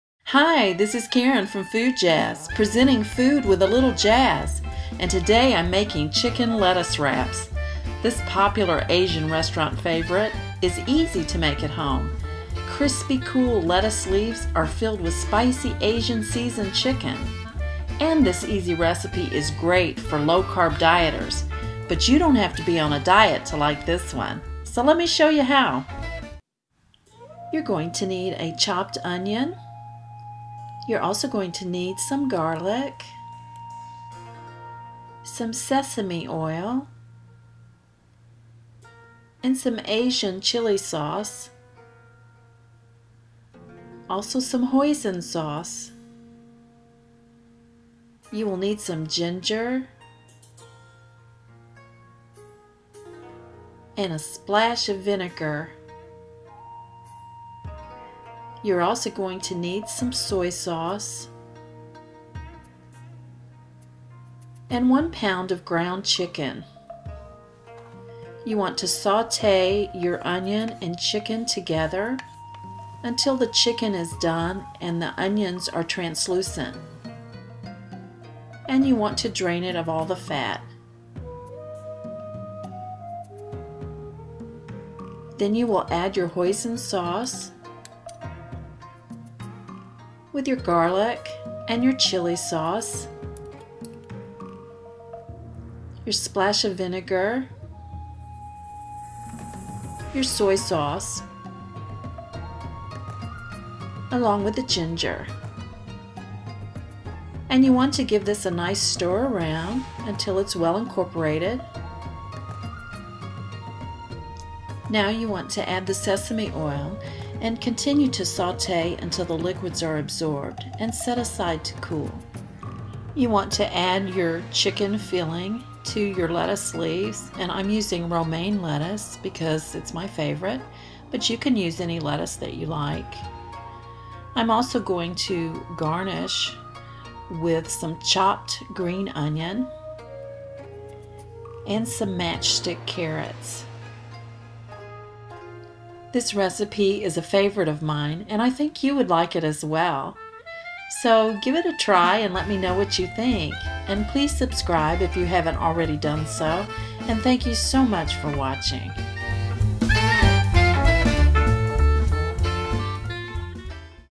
Chicken Lettuce Wraps
Chicken Lettuce Wraps- Crispy lettuce filled with asian seasoned chicken, topped with green onions, and carrots.

Ingredient list:

6 large lettuce leaves- Romaine or other
1 lb ground chicken
1 Small yellow onion- chopped
1 tblsp minced garlic
1 tblsp soy sauce
1/4 c hoisin sauce
1 tsp ground ginger
1 tblsp vinegar- rice or wine vinegar or plain
2 tsp asian chili pepper sauce- sriracha
2 tsp asian sesame oil
2 bunches green onion chopped
matchstick carrots for garnish